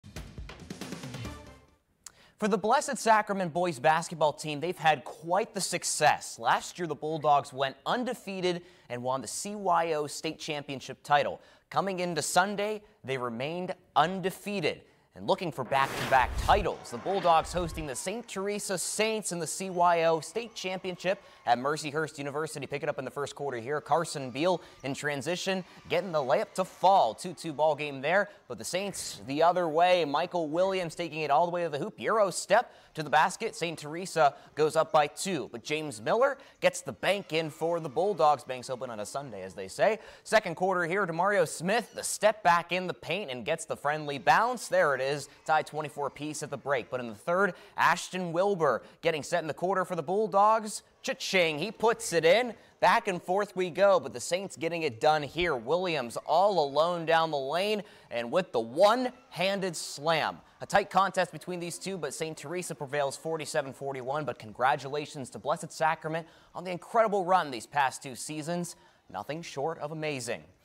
Blessed Sacrament Boy's Basketball Plays for CYO State Championship
The Bulldogs faced the St. Theresa Saints, looking for both back-to-back titles and undefeated seasons